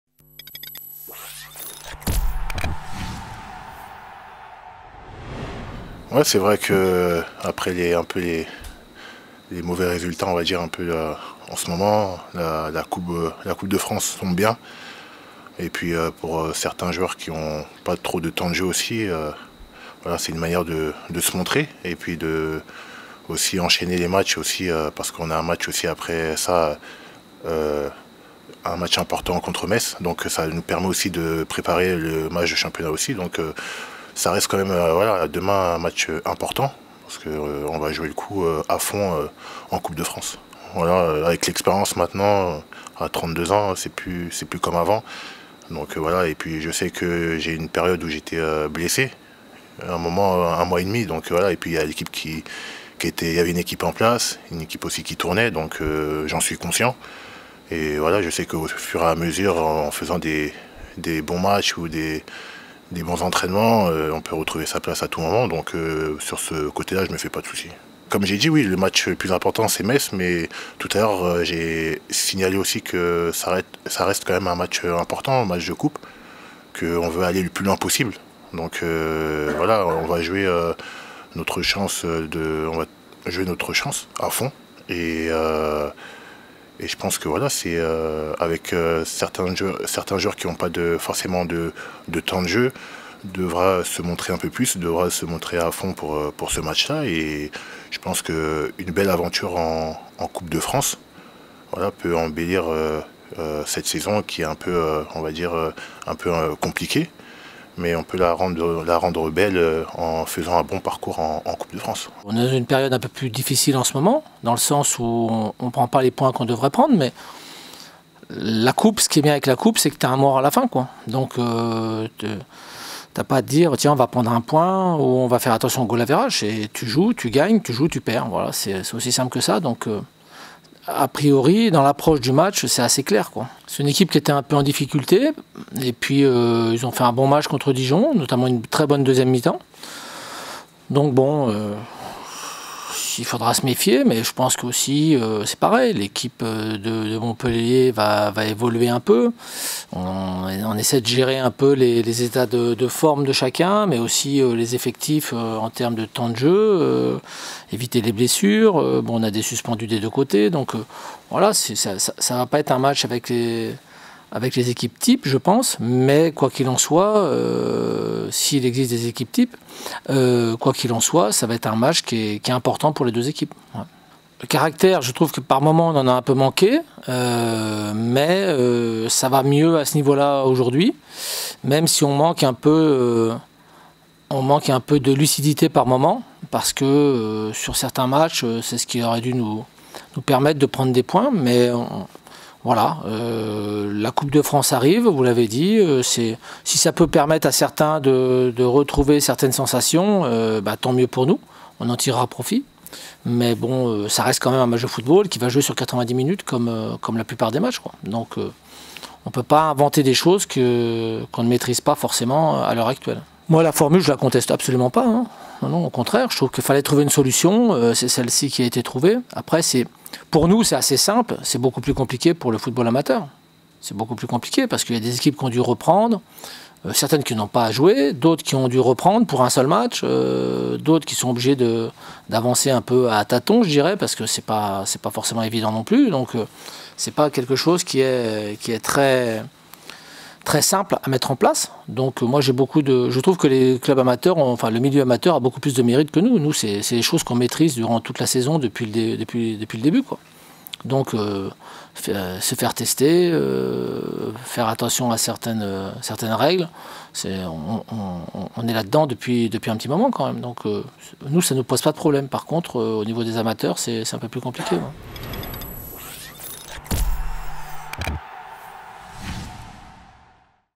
Racing-Montpellier HSC (1/32 CDF) : l'avant match
Avant de faire leur entrée en lice en Coupe de France, Thierry Laurey et Lamine Koné se sont exprimés devant la presse sur ce 32e de finale face à Montpellier.